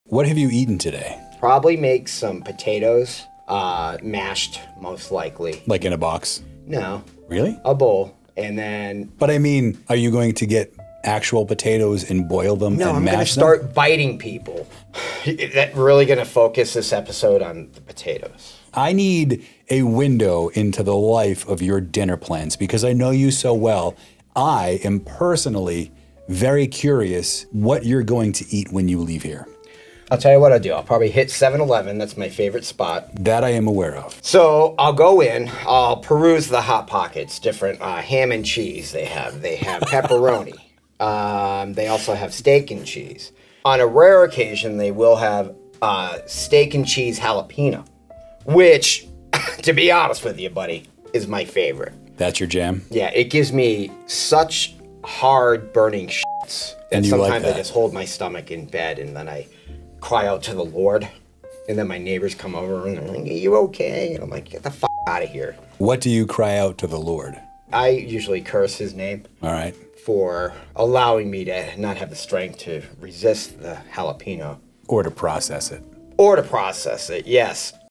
"Hot Pockets"
Paul has a love/hate relationship with Hot Pockets